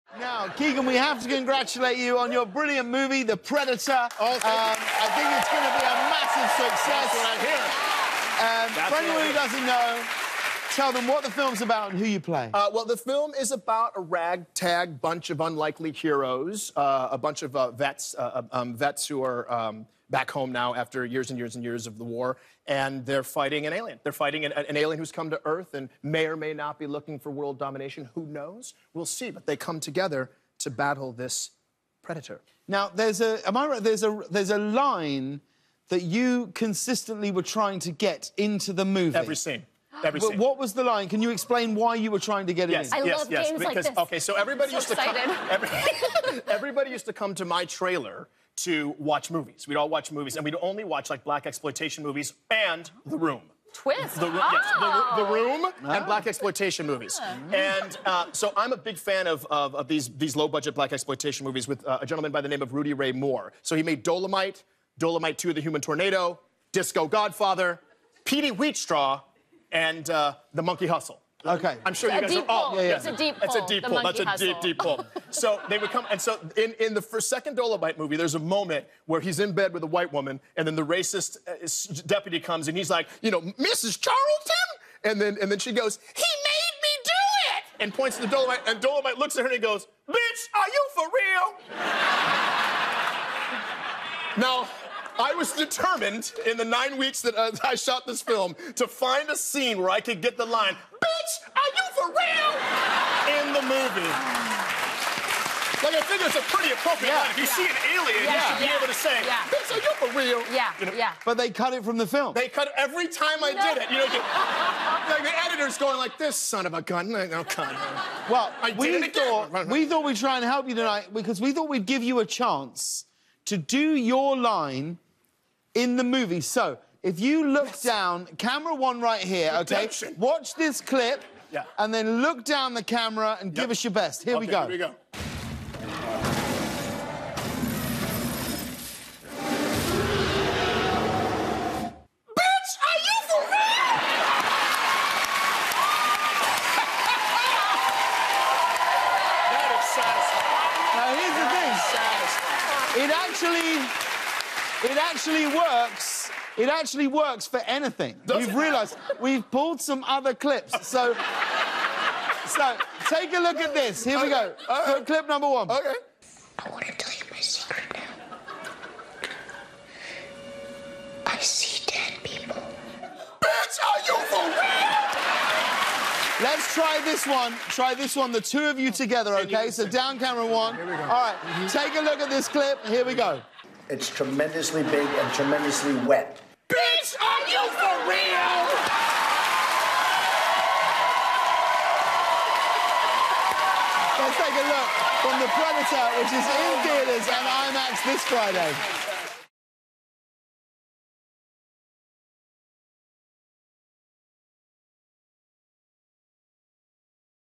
Keegan-Michael Key's Best 'Predator' Line Was Cut
When James asks Keegan-Michael Key about taking on his role in "The Predator" he learns there was a line Keegan tried to add in to the film that never made it, a line so perfect it can be used as a reaction to nearly anything.